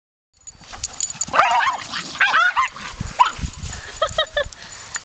Gizmo is like a Badger
small baby gizmo takes on a doberman that bothers him all the time.